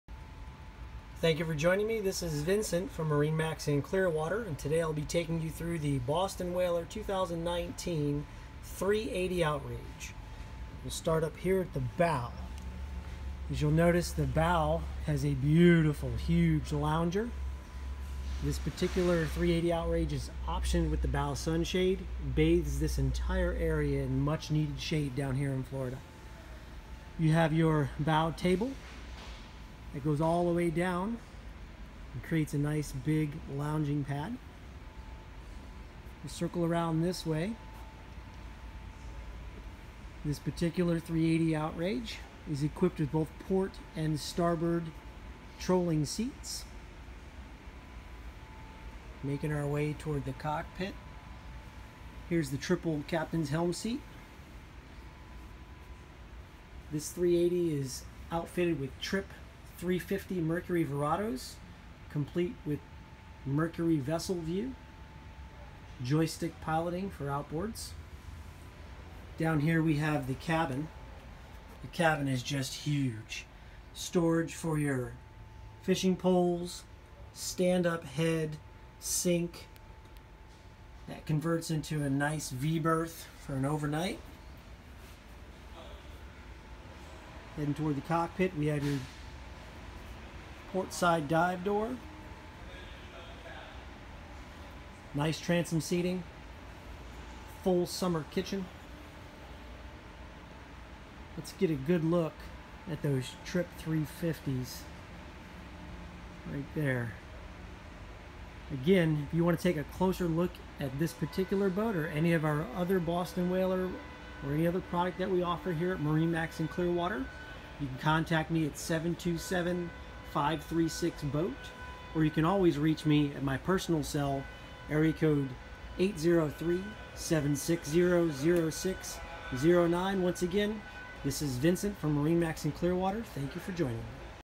2019 Boston Whaler 380 Outrage Boat For Sale at MarineMax Clearwater
The 380 Outrage delivers a full-spectrum, pinnacle experience, performing beautifully as an offshore fishing platform, an entertaining vessel, a yacht tender or all of the above.